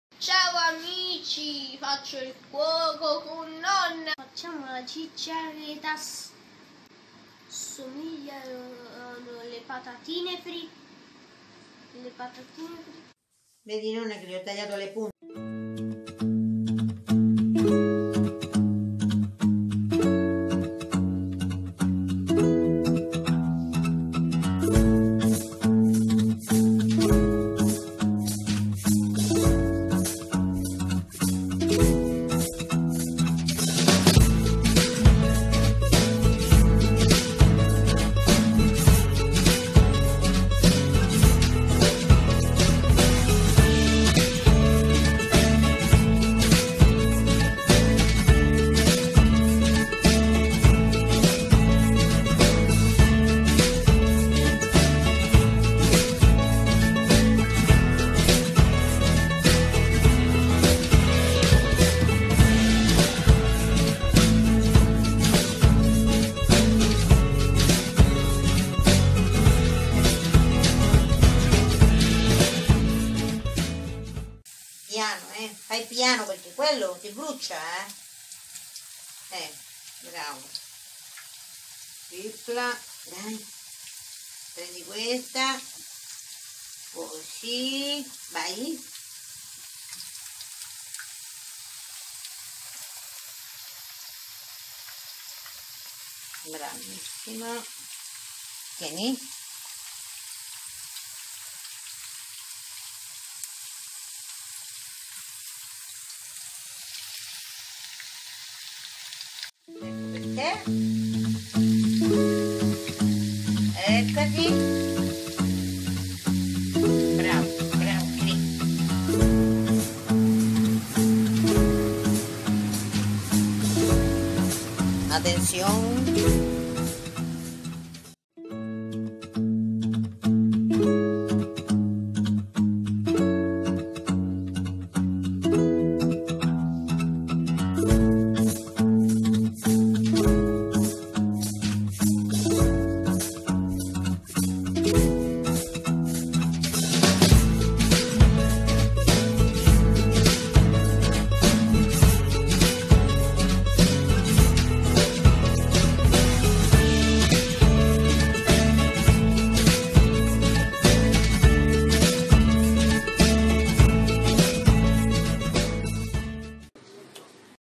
Cucino con nonna.CHICHARRITAS CUBANAS. Autismo.Autonomie e condivisione.
Mi piace tanto cucinare con la mia nonna  perche mi rende autonomo , utile ed è un ottimo momento per  imparare a condividere e rispettare i turni . Oggi facciamo  le "Chicharritas " così chiamate a Cuba.
Pronti, partenza , viaaaa. 😋😍 Besos😘😘

I commenti sono stati disabilitati da Youtube  per la privacy dei minori ma potete scriverci sulla nostra COMMUNITY .
Grazie, a presto!!😊